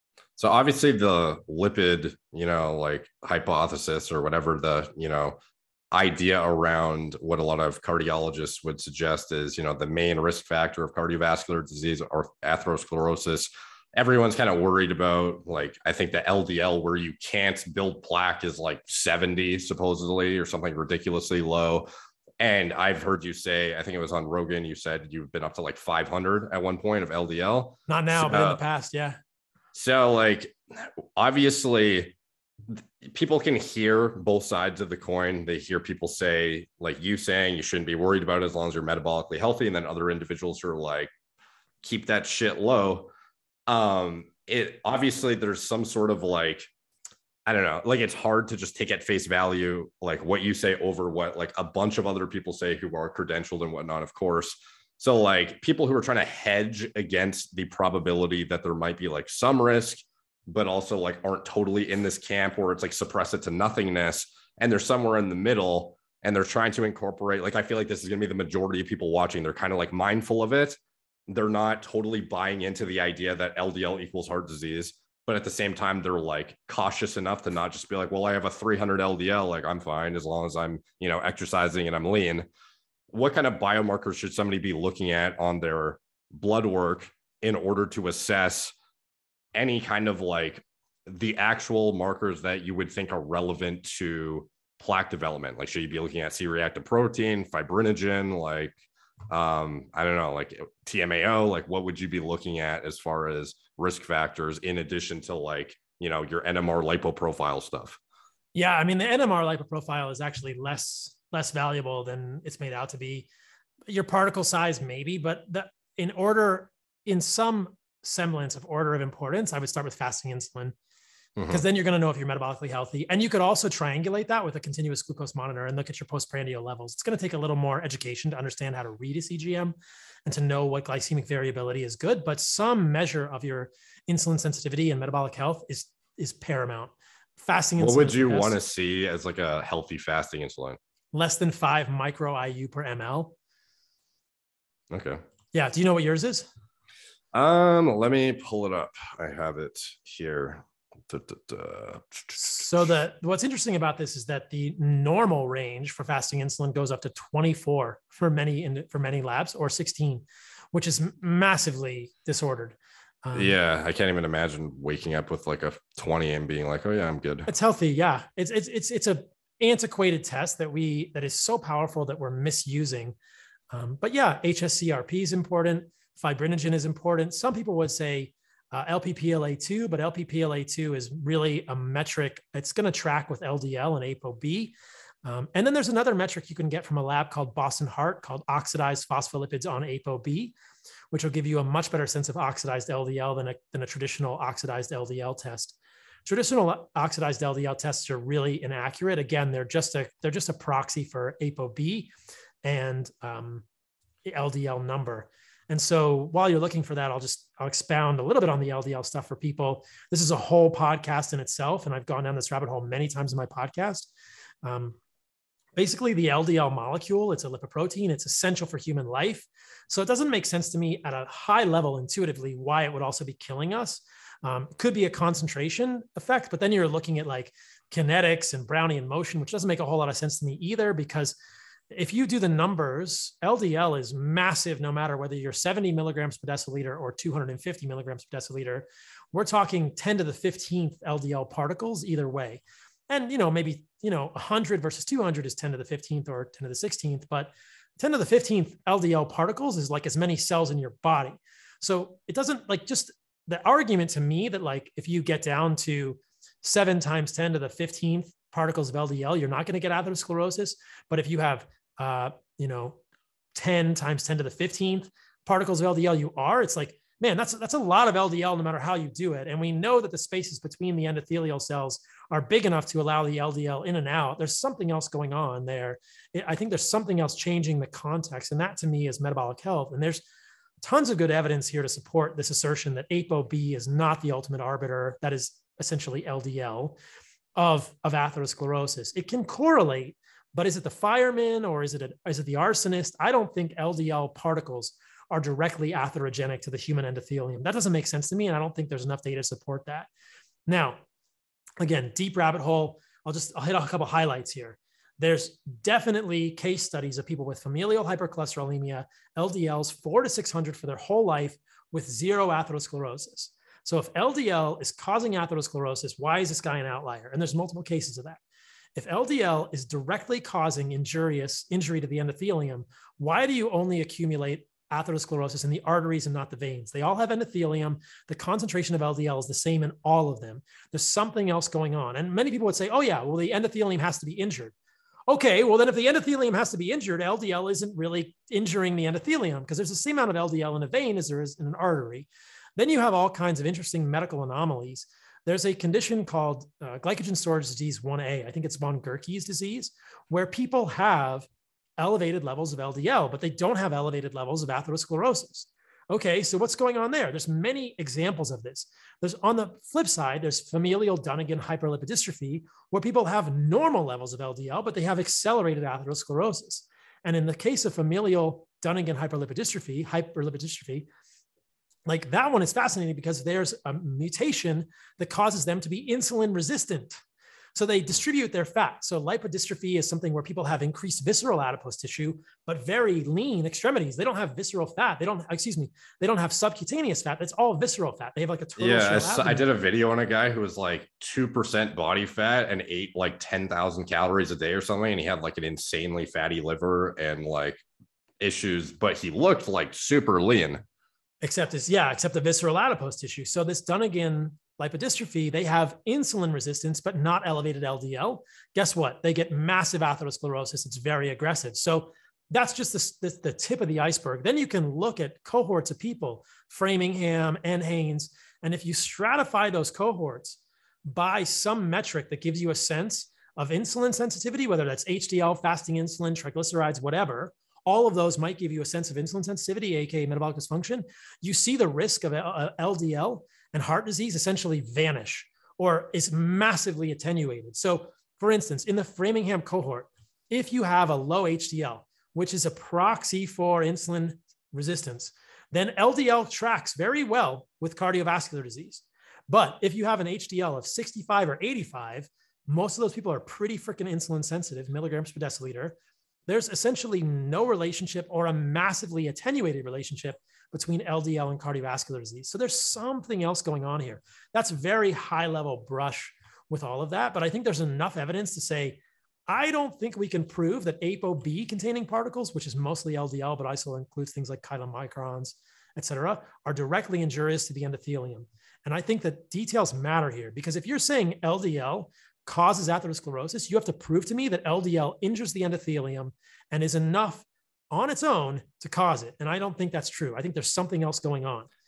Why high LDL cholesterol does not indicate metabolic dysfunction
Paul reviews his philosophy on LDL cholesterol and why he does not believe that high LDL injures the endothelium. He explains what to look for in terms of fasting insulin and HDL to indicate metabolic health and insulin sensitivity.